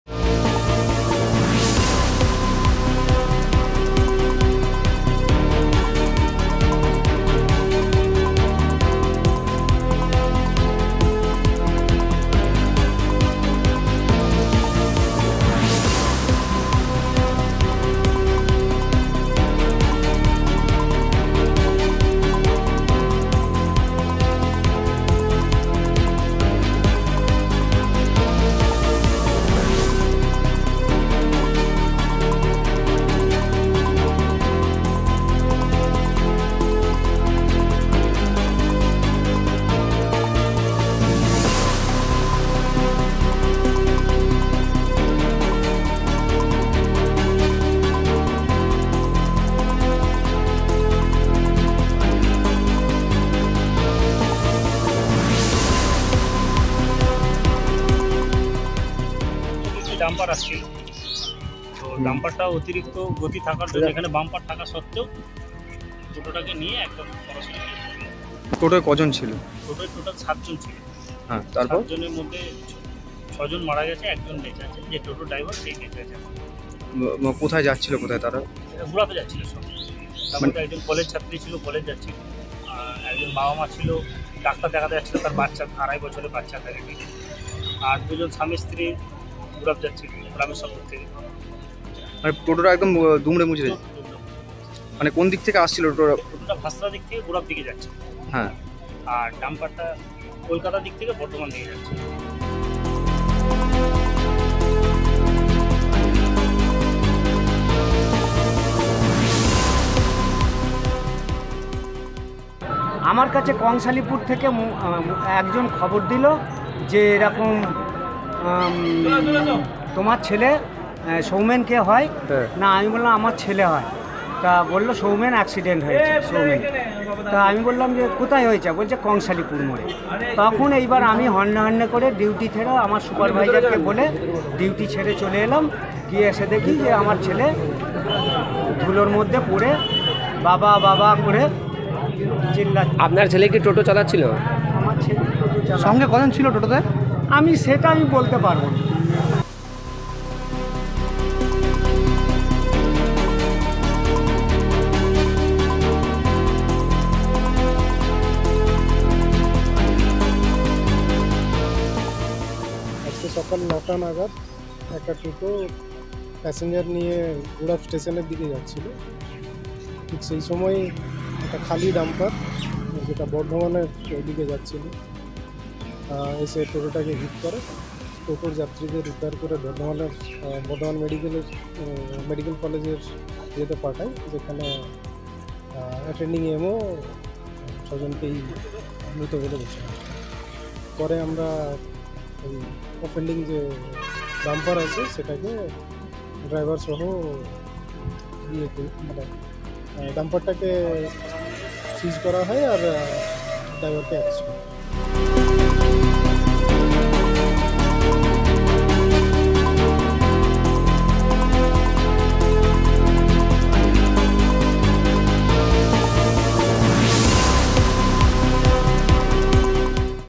Hooghly Accident: Child among 7 died in accident at Gurap | Sangbad Pratidin
Hooghly Accident: Child among 7 died in accident at Gurap after a dumper hits the Toto. The incident exactly happens at the Kangshari-more region of Gurap.